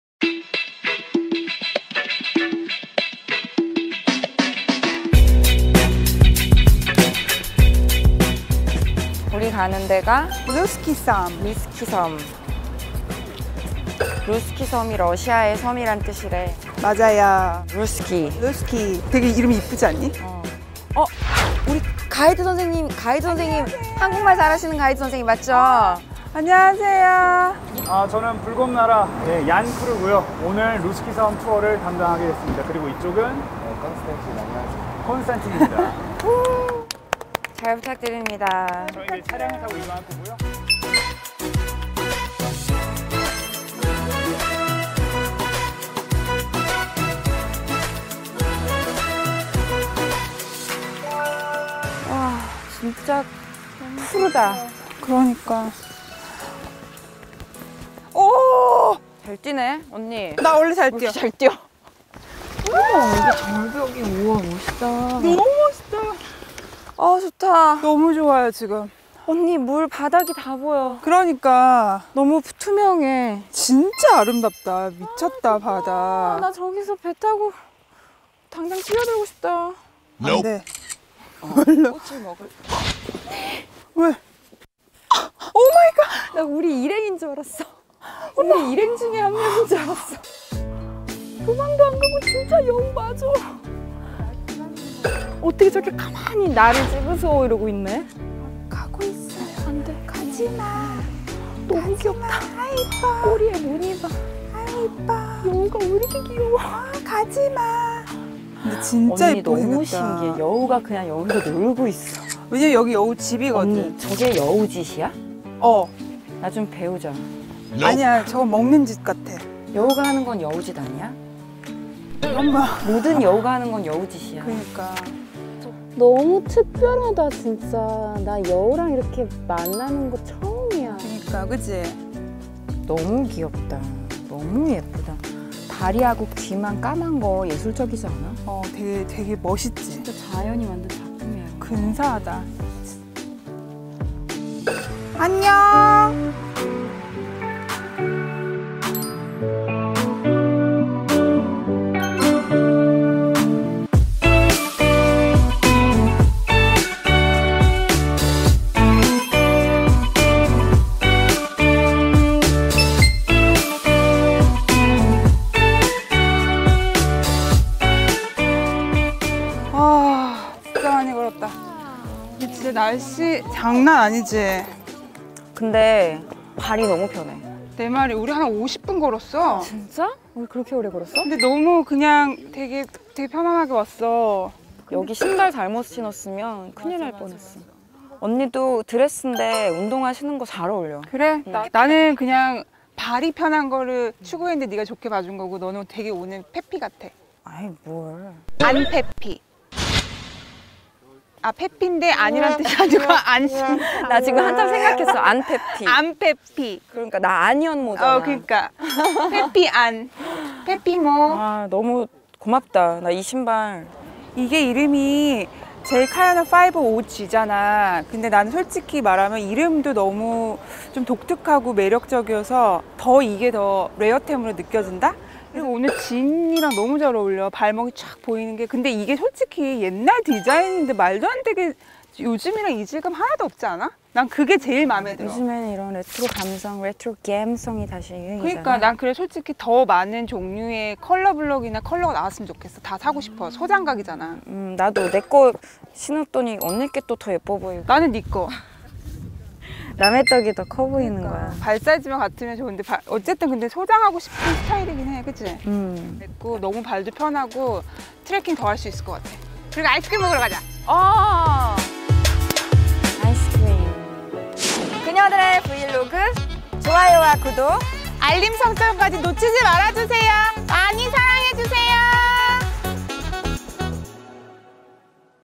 블라디보스톡 여행 브이로그 EP.07 야생 여우 만나본적 있나요? 블라디보스톡 최고의 셀럽 만날수 있는 루스키섬 트레킹 Vladivostok Russkii travel vlog
여자들을 위한 대리만족 여행 프로젝트 그녀들의 브이로그!!

세번째 여행 게스트는
엄친딸도 이런 엄친딸이 없다 만능 엄친딸 안현모!!

현모랑 함께 떠나는
한국에서 가장 가까운 유럽 블라디보스톡 여행

마지막 에피소드는

인스타 갬성의 자연경관이 너무나도 좋은
블라디보스톡 최고의 스팟 루스키섬

좋은 자연경관에서 트레킹하면서 멋진 스팟에서 사진도찍고
날씨가 좋으면 루스키섬 마스코트 여우도 만날수 있는데

그녀들의 브이로그 촬영때는 만났습니다!!
너무너무 사랑스러운 여우 친구들!!!

블라디보스톡 가시면 꼭 트레킹하러 가보셔요!!

이번에피소드로 그녀들의 브이로그 블라디보스톡편은
마무리 되었습니다

다음에도 좋은 도시 소개시켜드릴테니